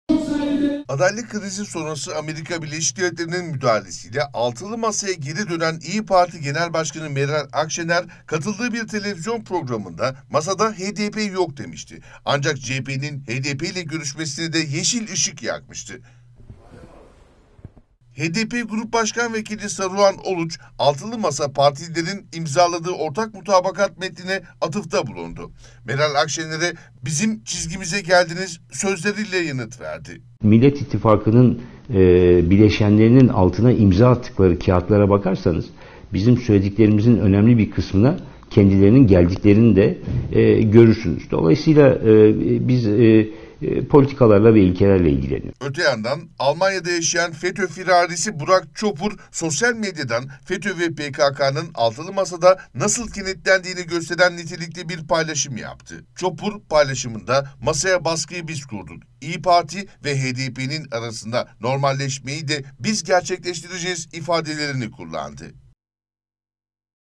HDP 'den Altılı Masaya sıcak mesaj: "Bizim çizgimize geldiniz"
HDP Grup Başkanvekili Saruhan Oluç'tan altılı masaya sıcak mesajlar geldi. Oluç, “Millet İttifakı bileşenlerinin imza attıkları kağıtlara bakarsanız, bizim çizgimize geldiklerinin görürsünüz” dedi. Öte yandan FETÖ firarisi Burak Çopur, sosyal medyadan yaptığı açıklamada, "Masaya baskıyı biz kurduk. İYİ Parti ve HDP’nın arasında normalleşmeyi de biz gerçekleştireceğiz." ifadelerini kullandı.